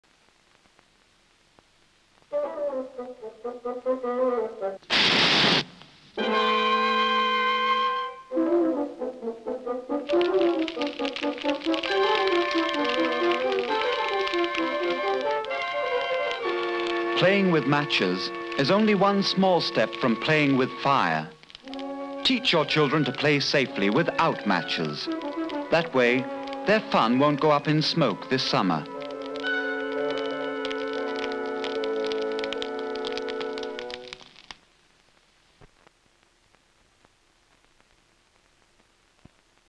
'Learn to live with fire' advertisements (3 of 5: 1 minute).
Examples of Forests Commission Victoria (FCV) media advertisments on fire preparation, survival and safety (+ sound).
Early 1970s - produced for TV and cinema use.